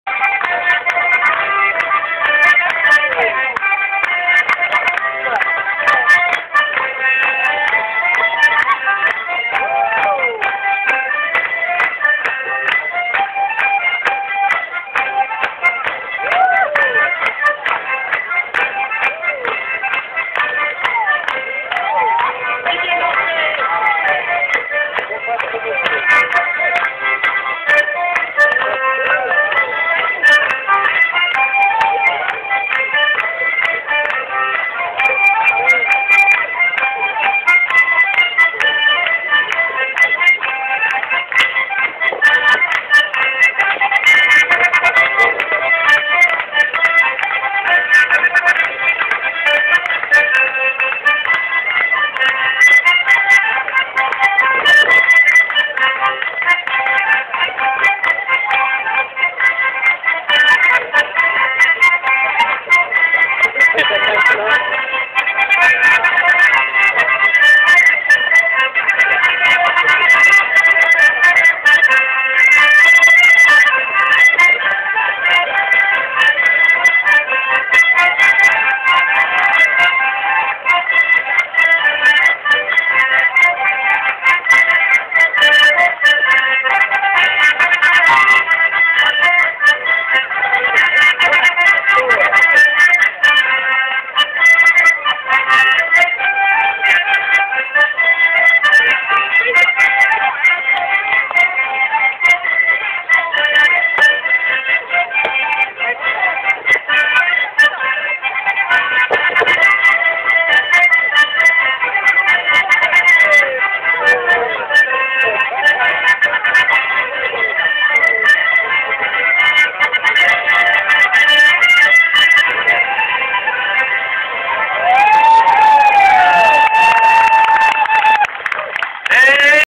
Leigh Folk Festival 2008
some dancers...thought it looked like a well dressed conga line :)
Loved the outfits. fun to watch.